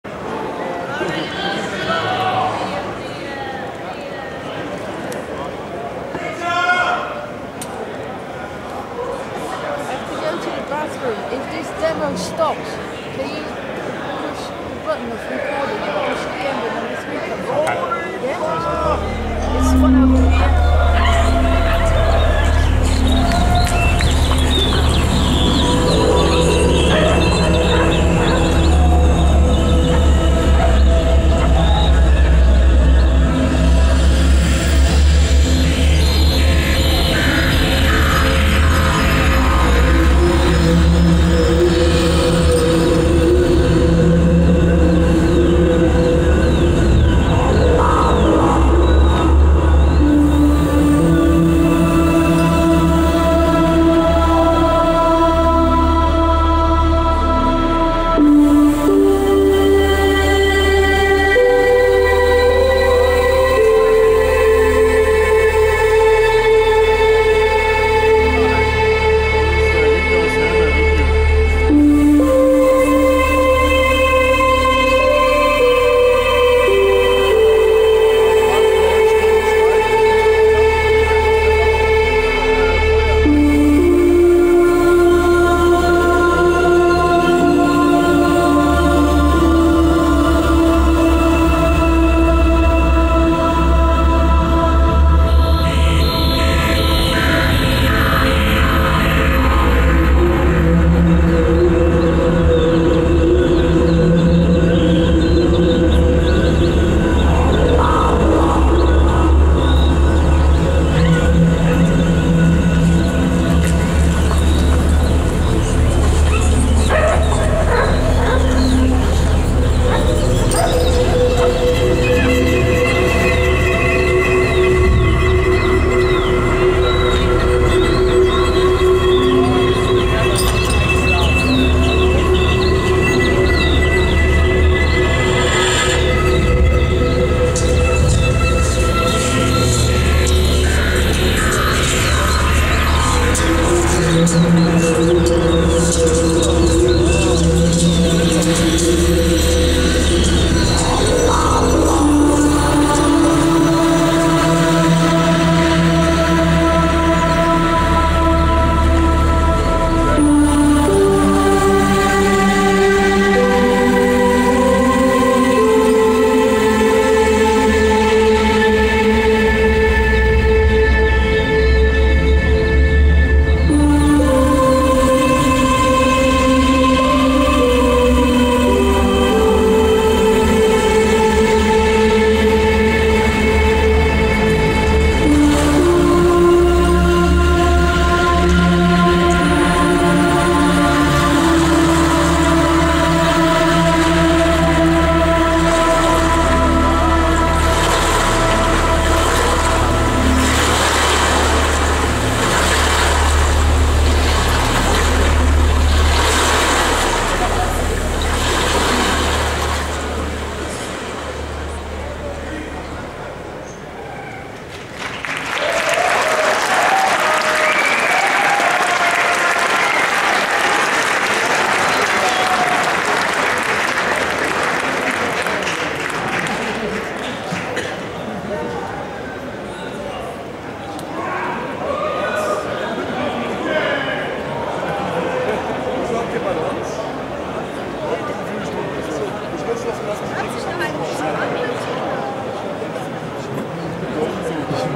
Revision 2012 - PC Demo Compo - Spring Brought Us Dry Withered Flowers (Live Footage)
Revision 2012 - PC Demo Compo - Spring Brought Us Dry Withered Flowers by Brainstorm and Traction (Live Footage)